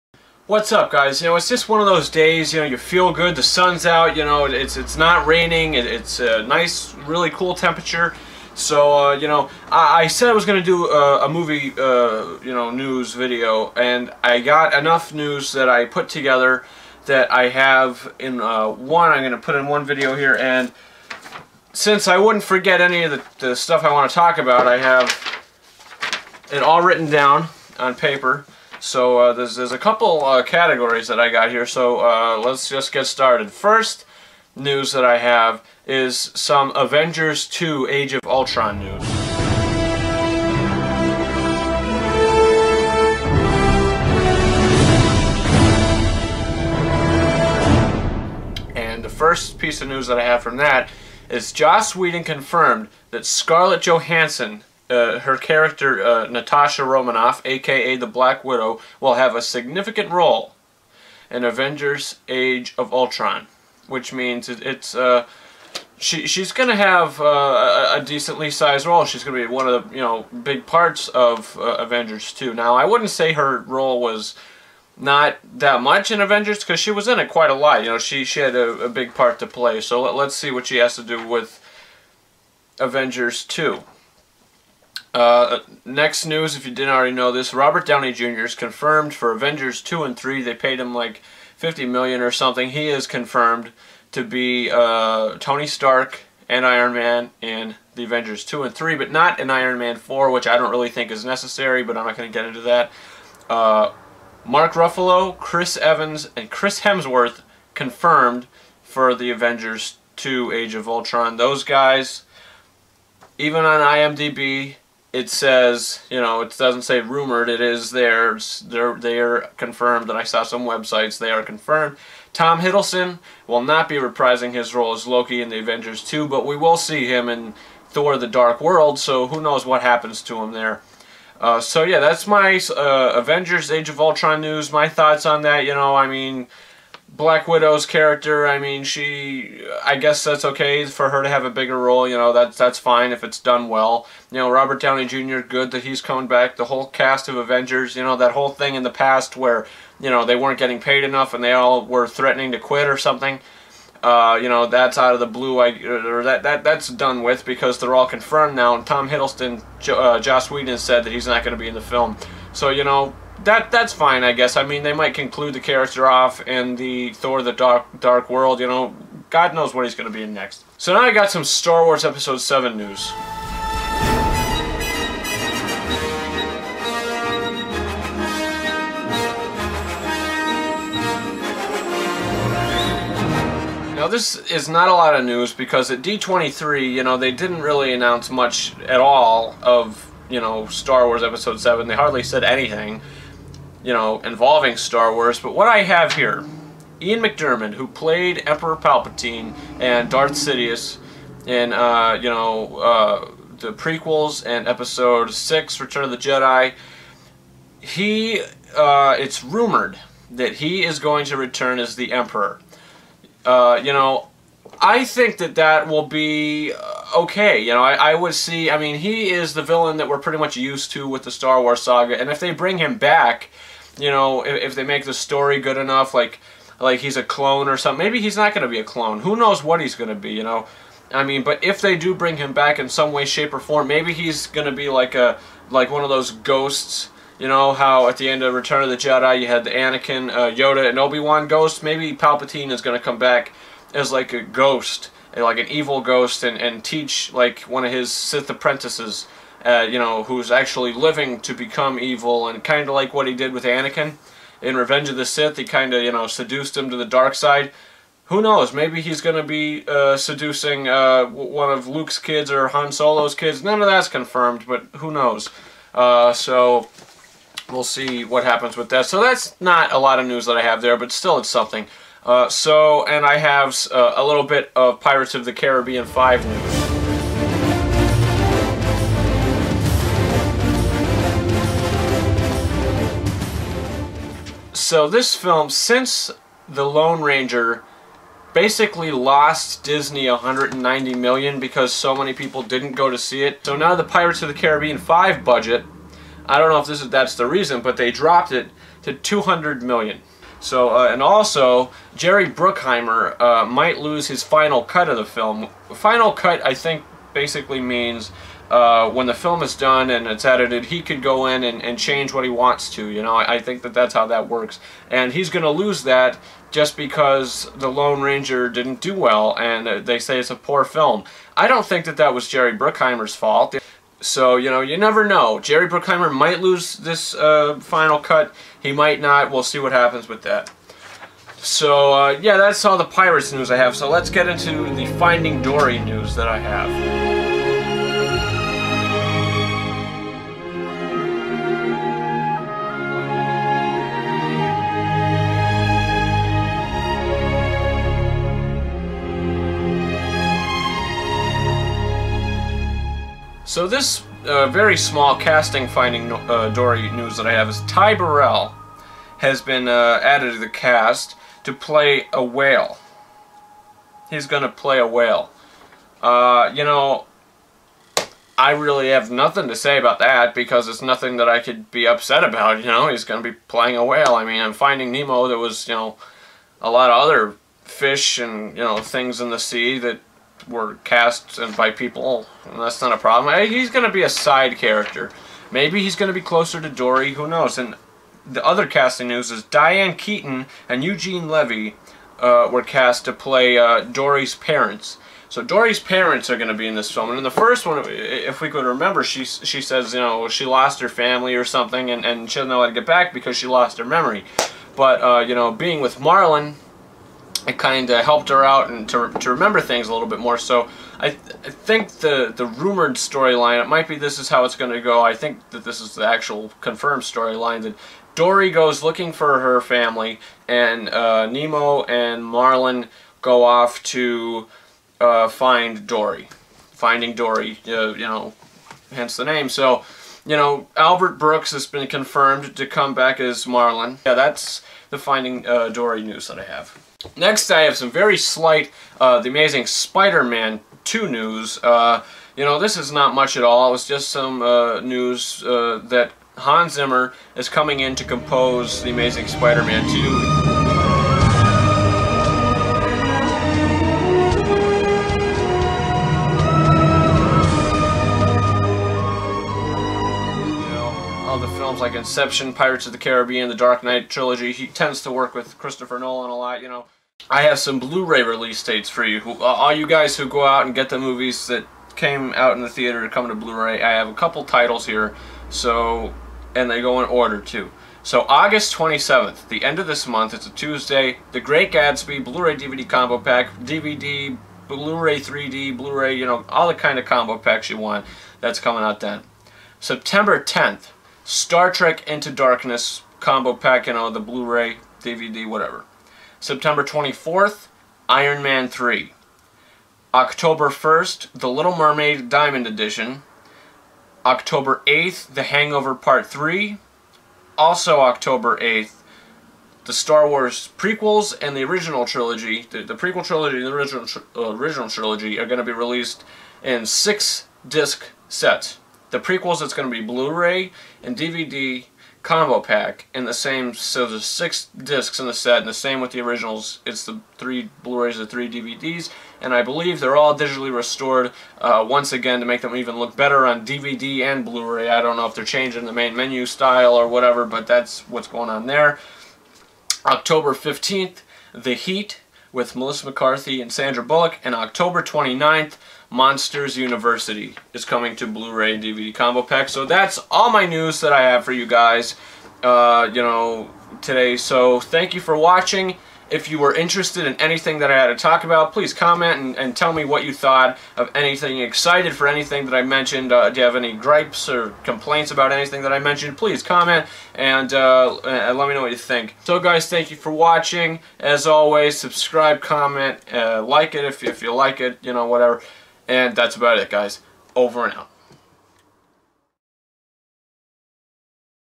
Movie News:Star Wars 7, Avengers 2 + more
I talk about some of the latest movie news then give some Blu-Ray release dates!